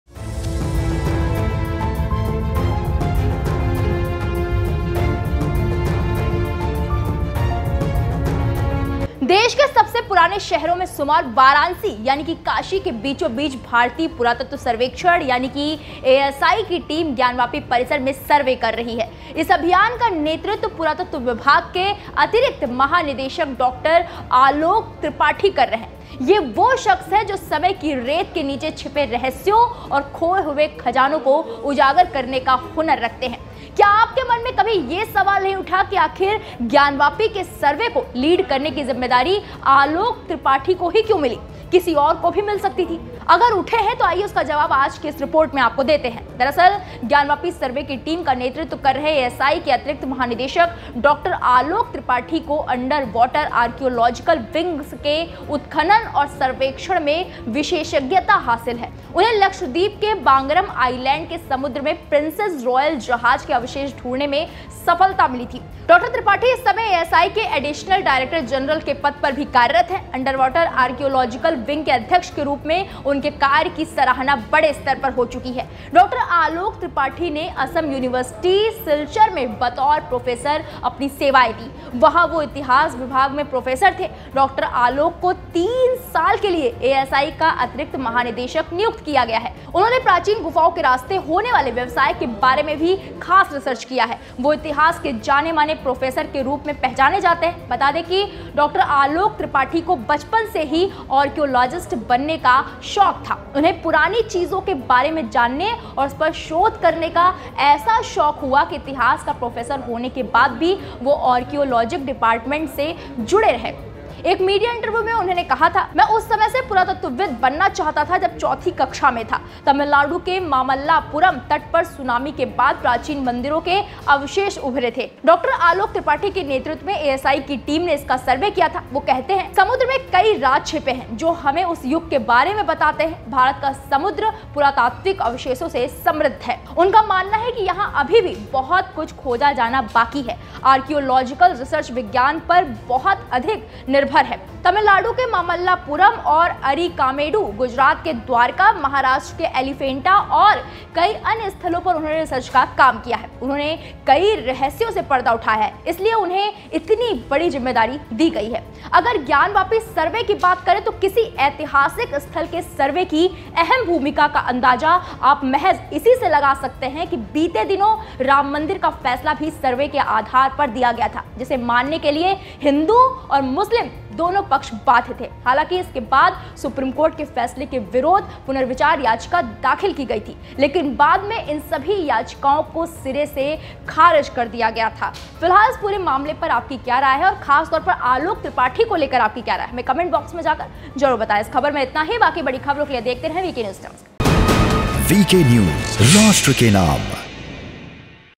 ASI सर्वे को लीड करने वाले अलोक त्रिपाठी हैं कौन ? | Alok Tripathi | Gyanwapi ASI Team Head
डॉक्टर आलोक त्रिपाठी असम यूनिवर्सिटी सिलचर में इतिहास के प्रोफेसर भी रह चुके हैं। वह अंडरवॉटर आर्कियोलॉजिकल विंग के अध्यक्ष रहे हैं और इस दौरान उनके काम के लिए उनकी खूब तारीफ भी हुई है। डॉक्टर आलोक ने पुरातन गुफाओं के रास्ते होने वाले व्यापार के बारे में भी स्टडी की है। वह बचपन से ही आर्कियोलॉजिस्ट बनना चाहते थे।

Dr. Alok Tripathi has also been a professor of history at Assam University Silchar. He has been the President of the Underwater Archaeological Wing and during this time he has been highly praised for his work. Dr. Alok has also studied the trade route through ancient caves. He wanted to become an archaeologist since childhood.